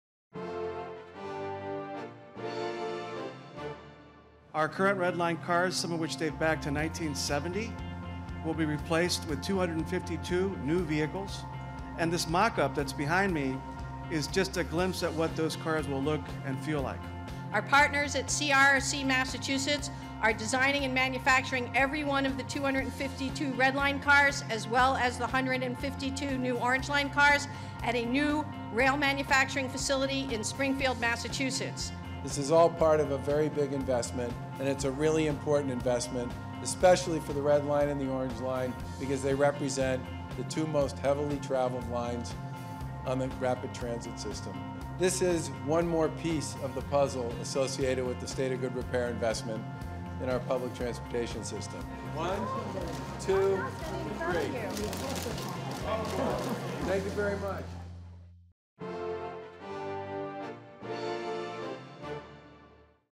Tour of the Red Line Car Display - Promo
Did you know that the average current Red Line subway car has traveled about 2.5 million miles? The MBTA has announced that they will be replacing all current Red Line cars, and 152 Orange Line Cars through the year 2022 to improve transportation and quality of life for people living and working in the city. Get a glimpse of what the future of rapid transit in Boston will look like with these newly designed trains.